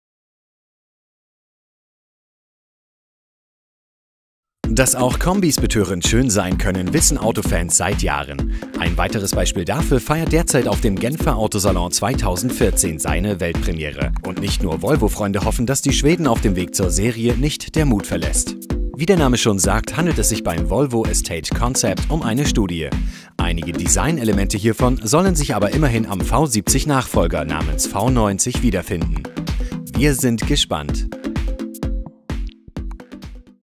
Volvo Concept Estate auf dem Genfer Autosalon 2014
Mit einer bildhübschen Kombi-Studie begeistert Volvo die Fans auf dem Genfer Autosalon 2014. Gebaut wird der Shooting-Brake aber leider nicht...